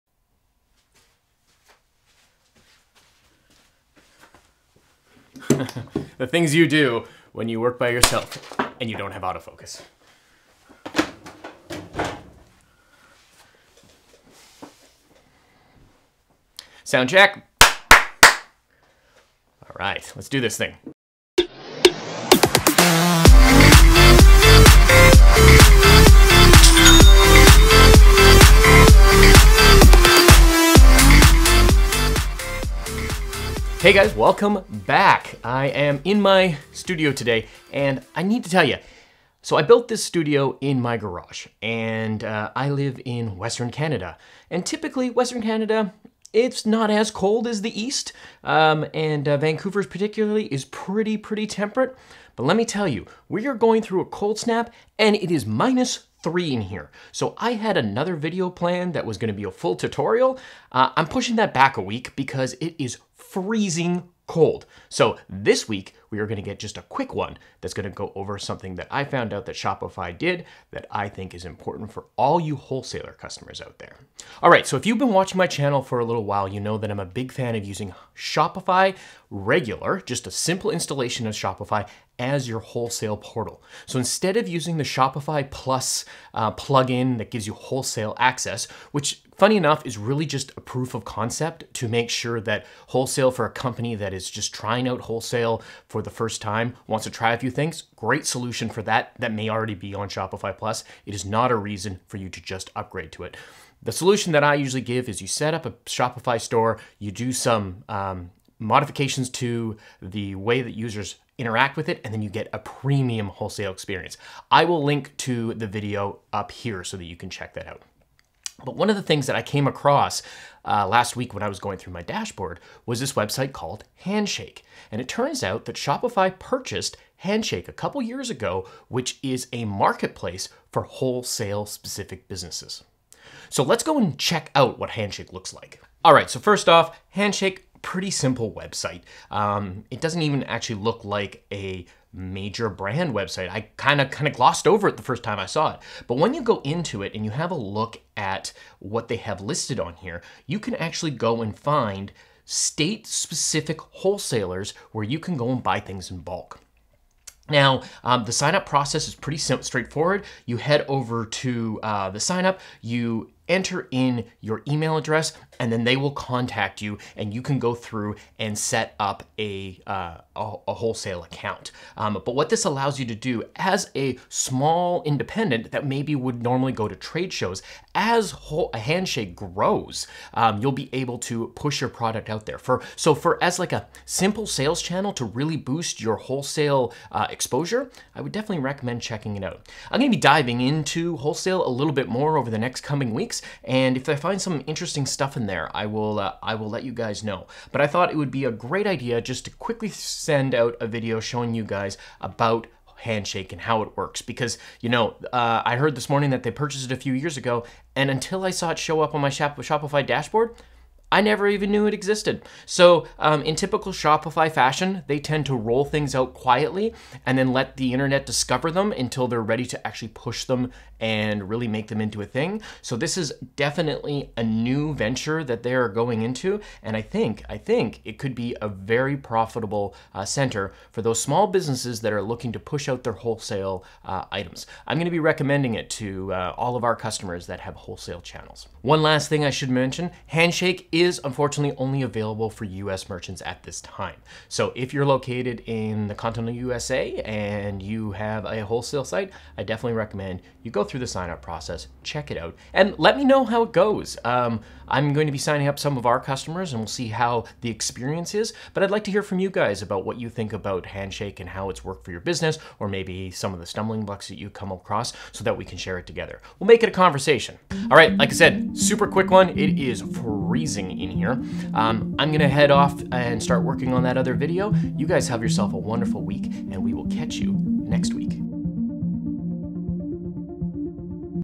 What is Shopify handshake ?
Wholesale maketplace provided by Shopify.